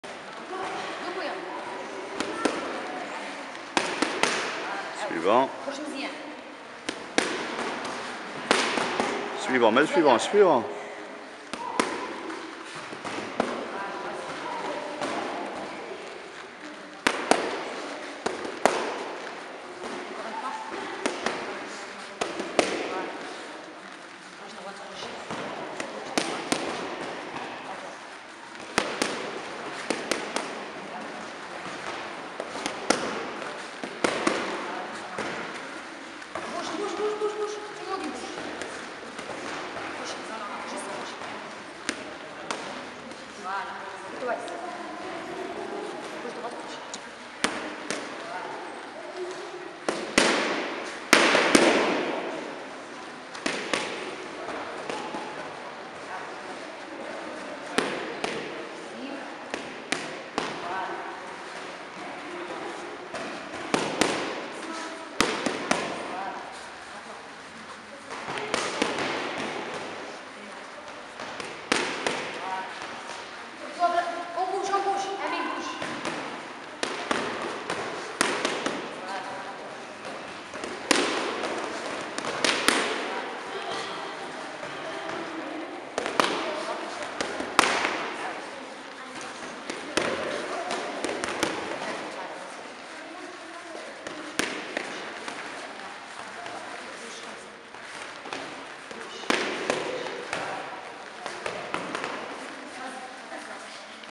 Boxe éducative Jah Boxing Academy
Travaille aux palettes des jeunes de la section BOXE éducative
de la JAH BXING ACADEMY BRUSSELS avec Sanae Jah
ou 0489/00.81.89.